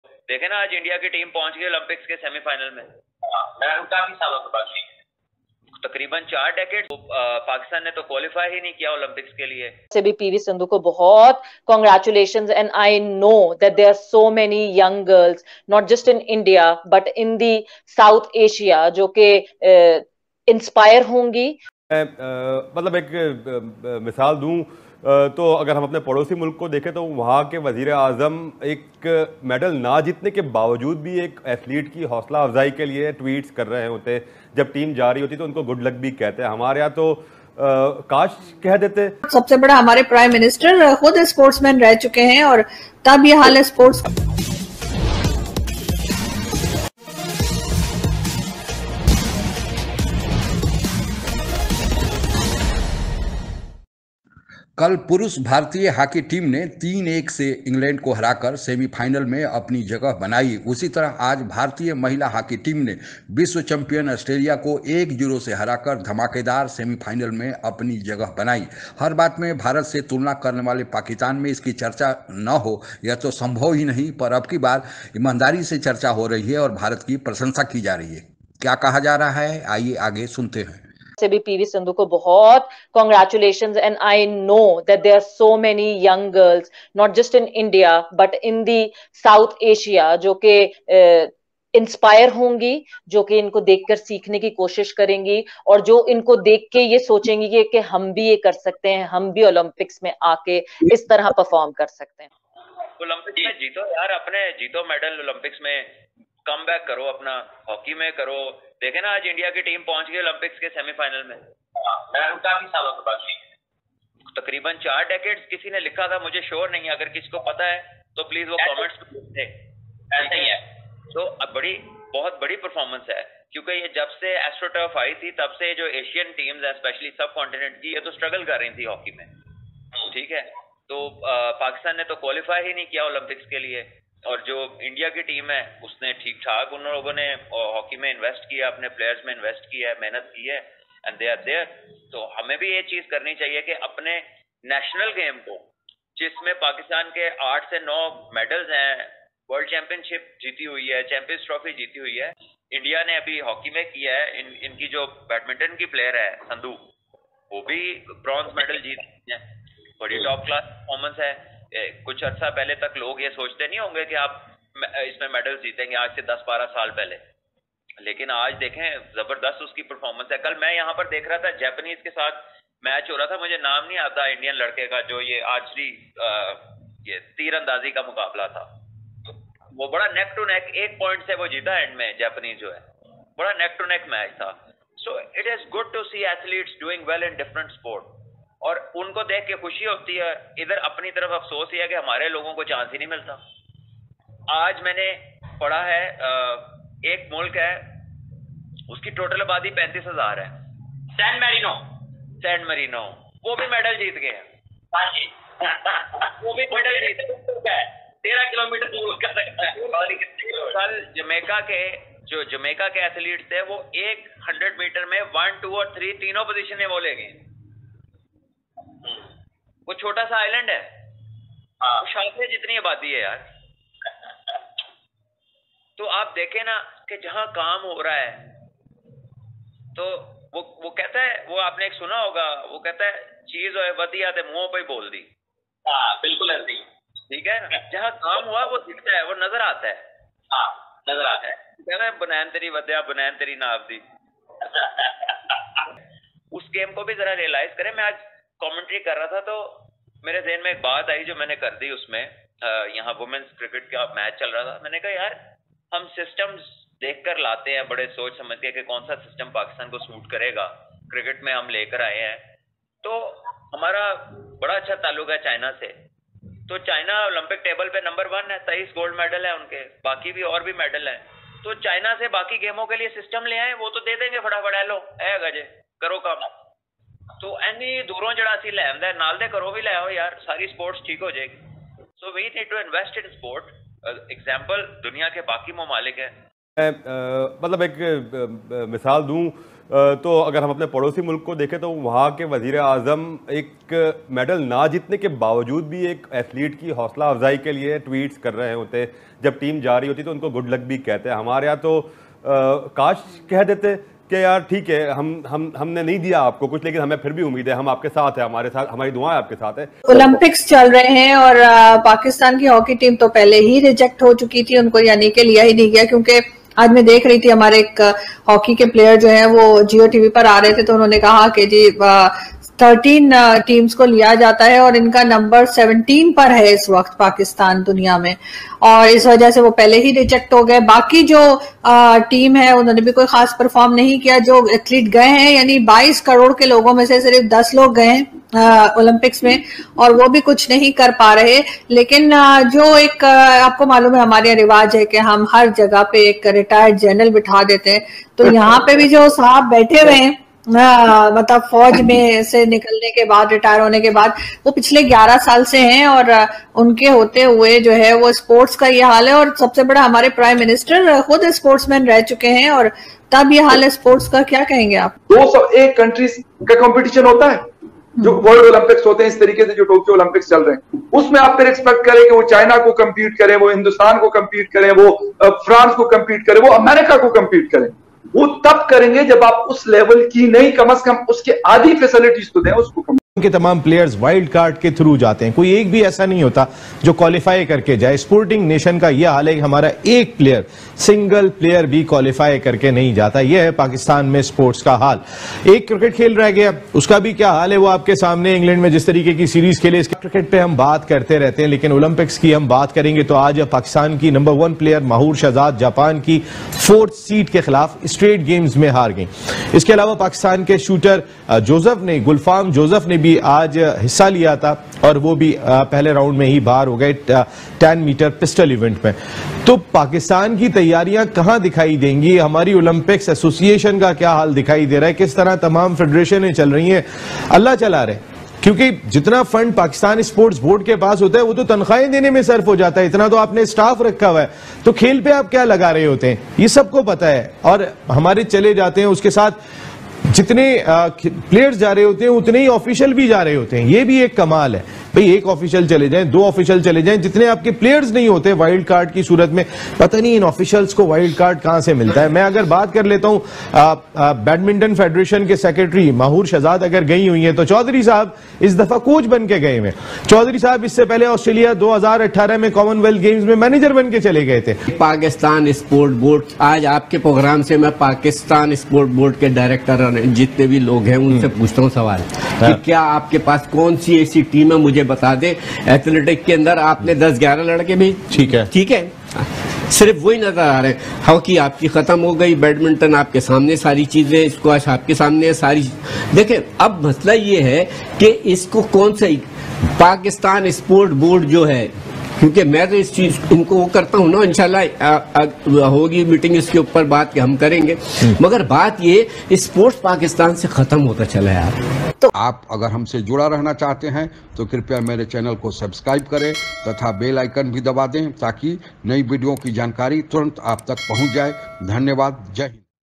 Tokyo Olympics | Performance of India 's PV Sindhu & Hockey Team praised by Pakistan | Pak Media
This video is reaction and analysis of Pakistan praising  India 's performance in Tokyo Olympics specially By PV Sindhu in badminton and by hockey teams reaching the semifinal in hIstoric display. .

Links of other Videos of Mine :

1.. श्री राम और हनुमान जी से सीखे सफलता के तीन सूत्र |3 Mantras of Success - RamcharitManas |Life lesson